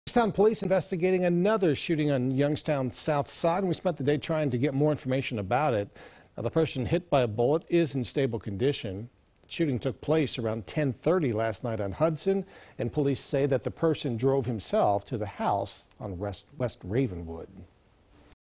Shooting on Youngstown’s south side leaves 1 hurt
It happened around 10:30 p.m. in the 3500 block of Hudson Ave., according to police. The victim then drove himself to a house on W. Ravenwood Avenue.